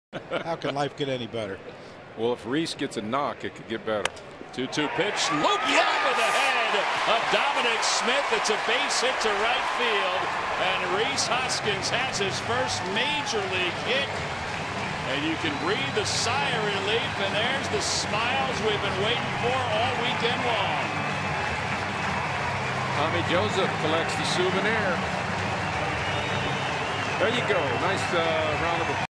NYM@PHI: Hoskins earns his first MLB hit
8/13/17: Rhys Hoskins lines a ball to right field in the bottom of the 5th inning to earn his first career base hit